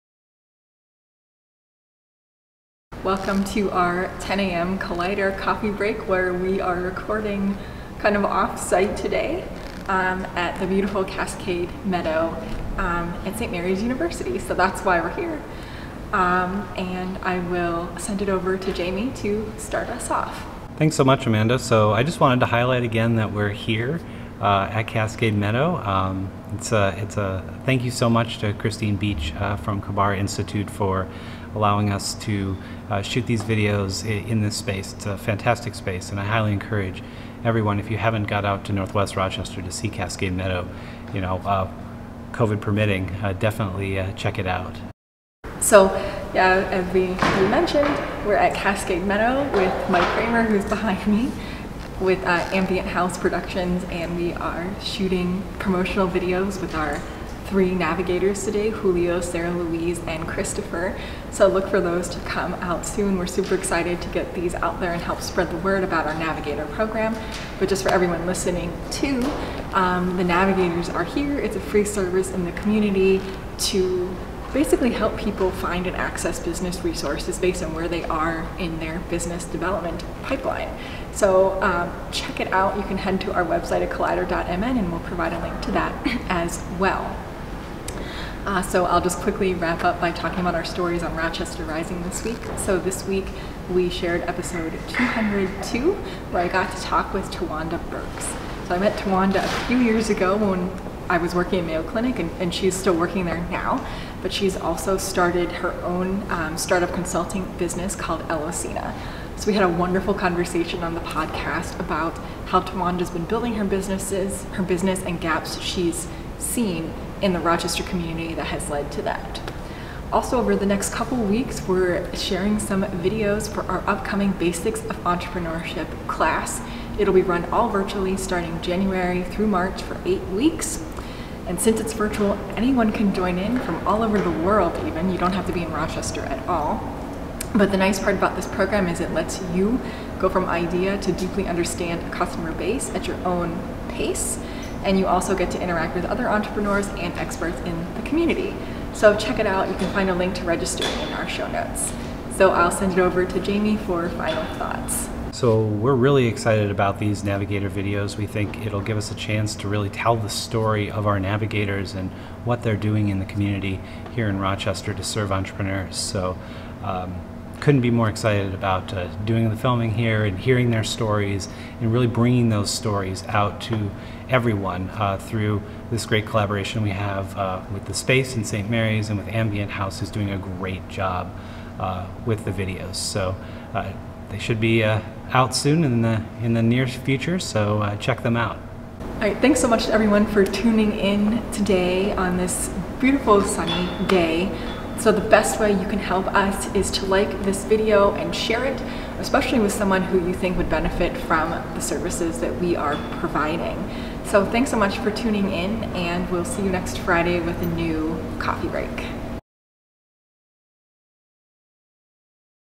December 4, 2020: 10AM Collider Coffee Break
Join us for our shortest coffee break ever to hear what we've been up to at Collider this week.

0:00 Start
0:21 COVID safe video shoot at Cascade Meadow
0:47 Video collaboration with Ambient House Productions
1:28 Rochester Rising podcast episode 202 with Elocina
2:01 Upcoming entrepreneurial education program
2:35 Telling the story of our Navigator program
3:10 Wrap up